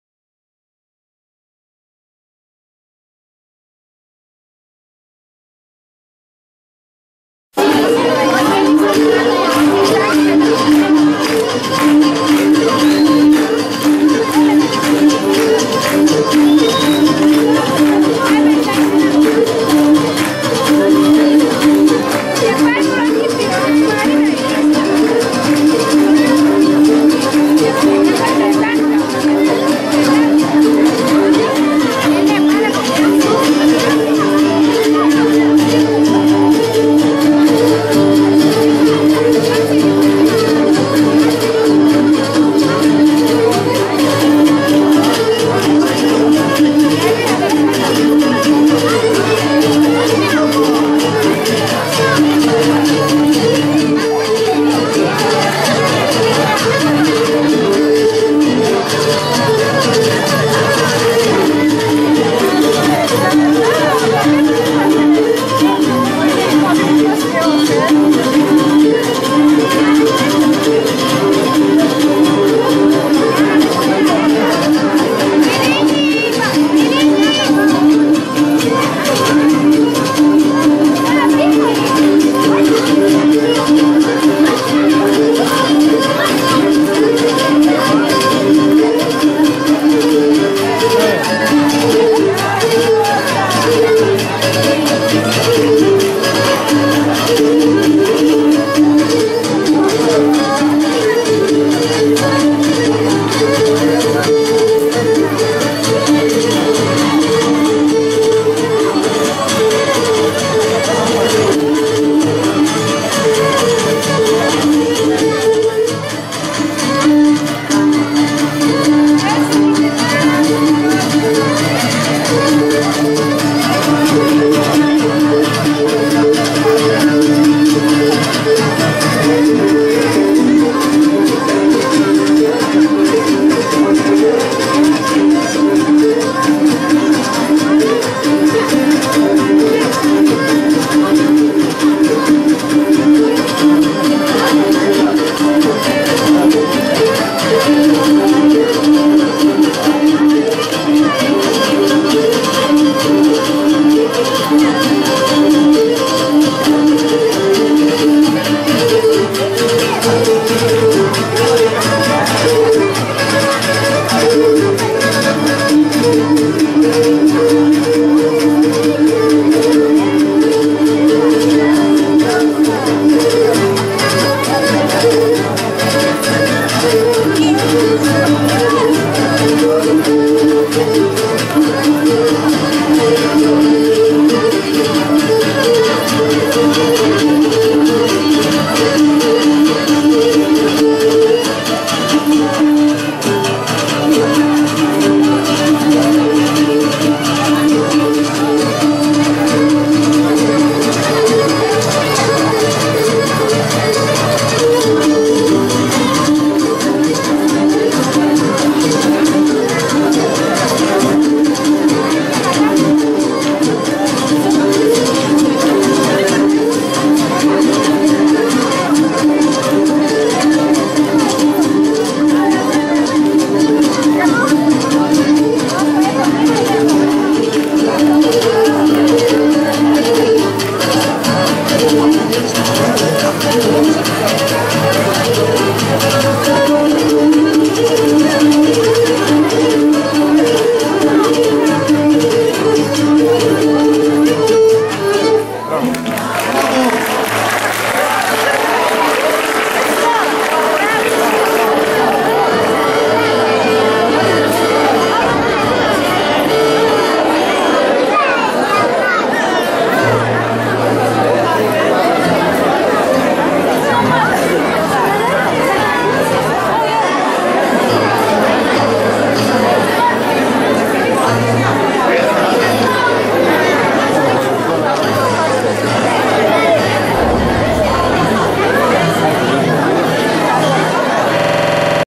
ΨΑΡΟΓΙΩΡΓΗΣ -ΑΝΩΓΕΙΑΝΟΣ ΠΗΔΗΧΤΟΣ 12/12/2011
ΓΙΩΡΓΗΣ&ΛΑΜΠΗΣ ΞΥΛΟΥΡΗΣ ΚΩΣΤΑΣ ΣΚΟΥΛΑΣ ΧΟΡΕΥΕΙ ΤΟ ΣΥΓΚΡΟΤΗΜΑ ΤΟΥ Λ.ΚΑΛΟΜΟΙΡΗ ..ΣΤΟ ΔΗΜΟΤΙΚΟ ΣΧΟΛΕΙΟ ΑΝΩΓΕΙΩΝ..ΣΕ ΣΥΝΕΡΓΑΣΙΑ ΜΕ ΤΟ ΣΥΛΛΟΓΟ ΓΟΝΕΩΝ ΤΩΝ ΑΝΩΓΕΙΩΝ. !